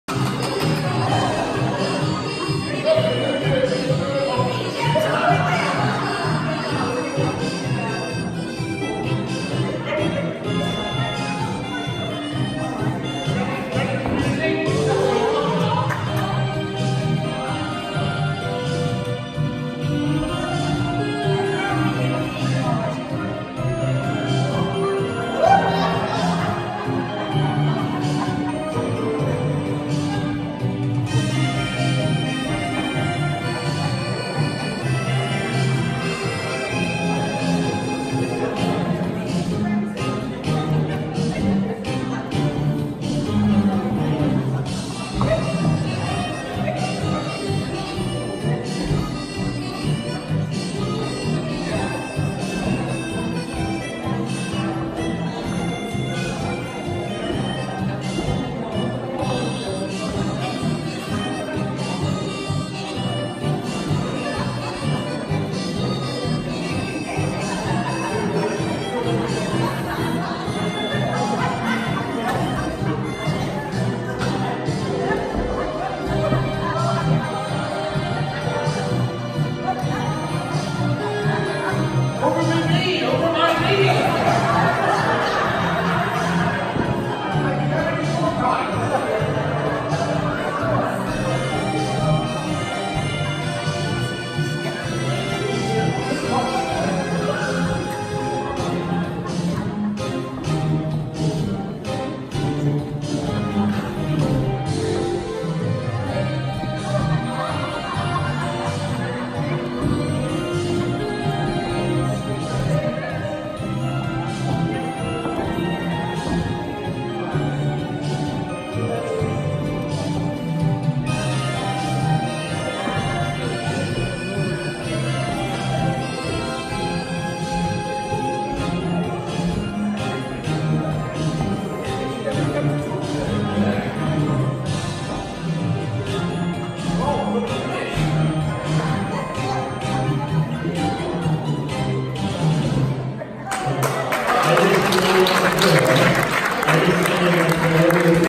Margo and Martin - Ballroom Tango
This is something that started a while ago when we started to teach Tango and it is now a bit of a party piece! So funny and I think the Tango is now Martin’s favourite dance!!